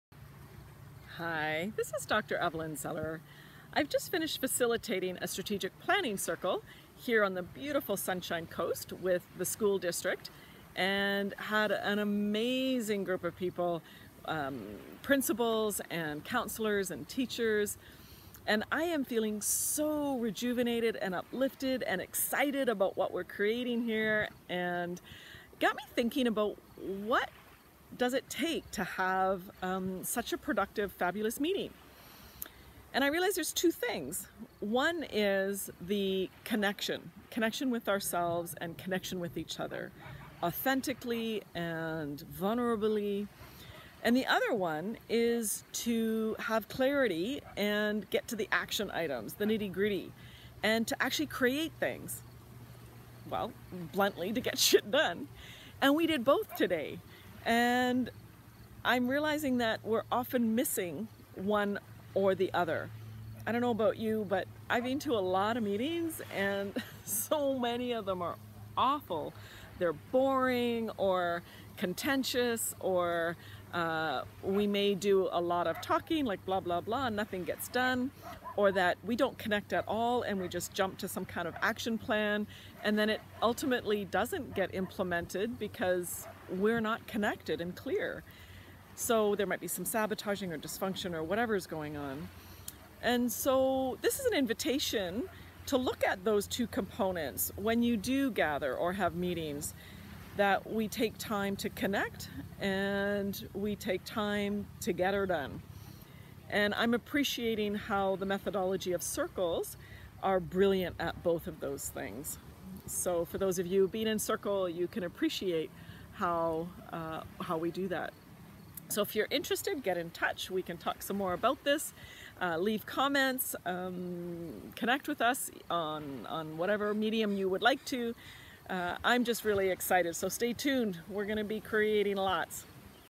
What's Required for Great Meetings?
Dr Evelyn Zellerer shares two components to having a fabulous, efficient, productive meeting. She just finished facilitating a Strategic Planning Circle with schools on the Sunshine Coast.